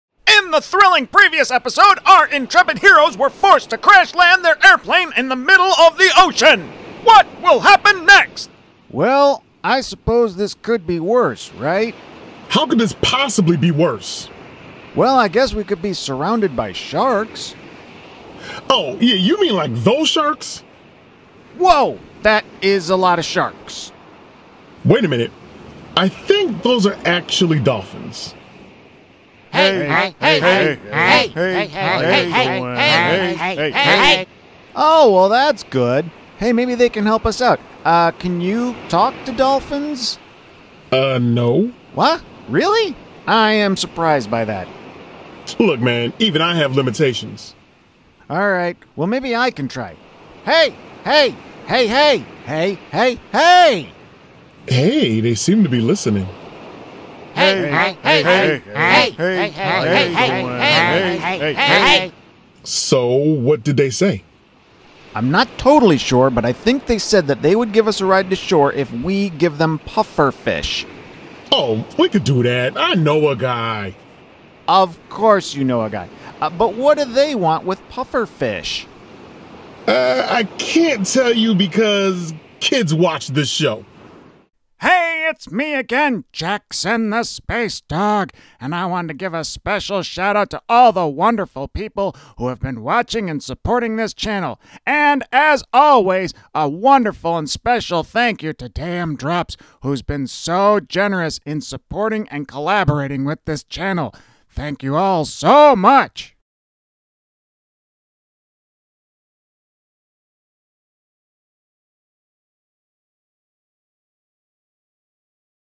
Babies Stranded on the Ocean
Two Brothers start their adventure together. They will get into all kinds of trouble along the way but first they need to figure out how they can work together. In this episode, Peter and Daymon have escaped from the adoption agency. But now they find themselves stranded on an airplane in the middle of the ocean.